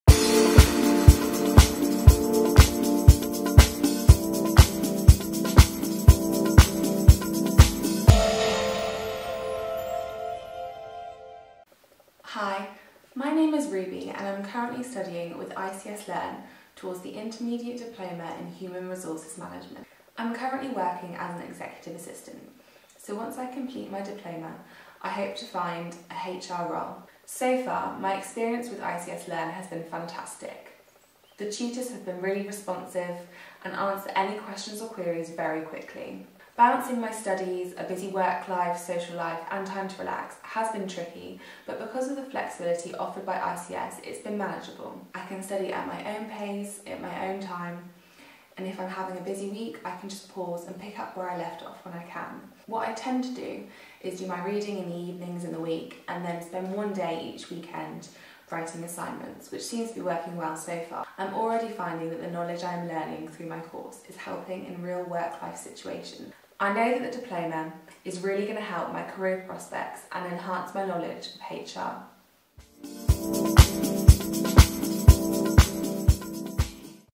Flexibility of Getting CIPD Qualified Online | Ruby's Review | ICS Learn
Ruby is learning online to achieve a CIPD qualification to help improve her HR career prospects. Taking advantage of the flexibility offered with online learning, Ruby is able to balance work and study.

Listen to Ruby's online learning strategy and the tips and tricks she uses to ensure the successful completion of an online CIPD qualification.